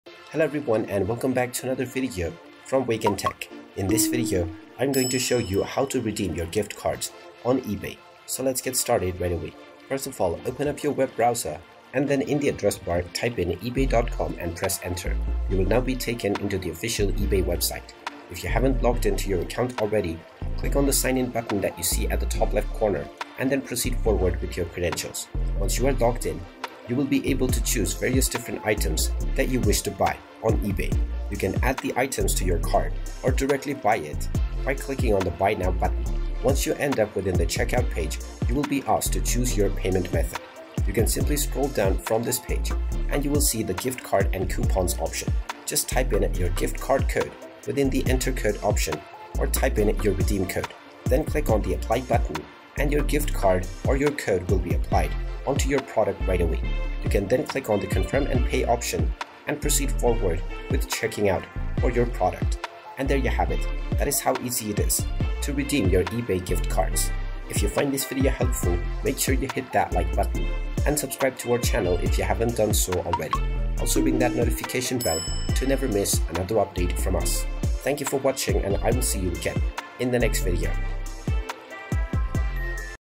How to Redeem Your Gift Cards on eBay? Utilize Your Gift Cards on eBay on PC 2024
Got an eBay gift card and ready to use it? In this video, we'll show you exactly how to redeem your gift cards on eBay so you can start shopping right away. Whether you’re new to eBay or just need a refresher, this easy-to-follow guide will ensure you get the most out of your gift card.